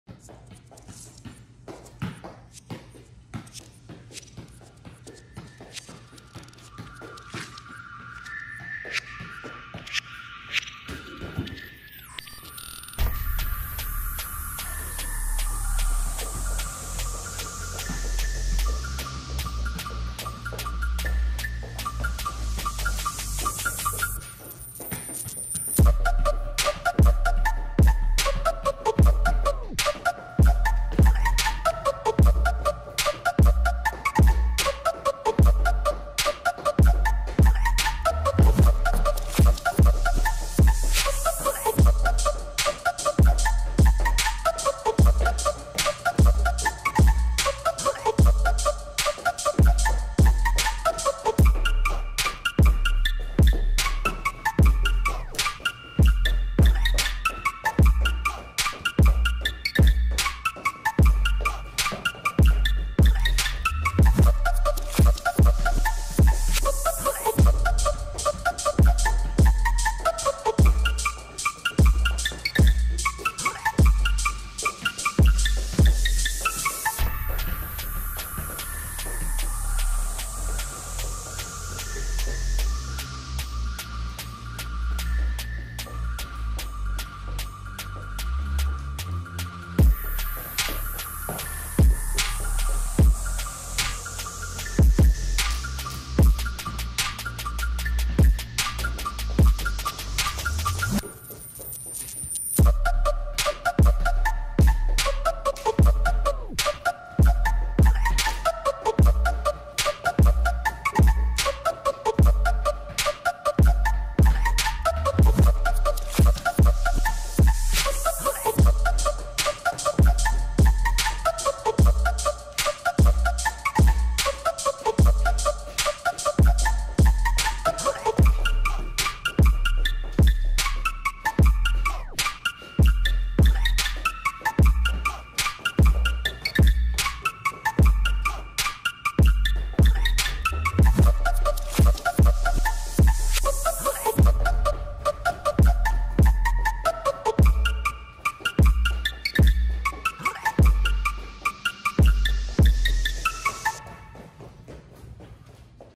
Full In-Home Technical Training Session - FDB Soccer
During times like this, it is hard to get out and train. Stay at home and keep training, no excuses allowed.

For this session, all you need is a ball and 3-4 cones. Nothing else.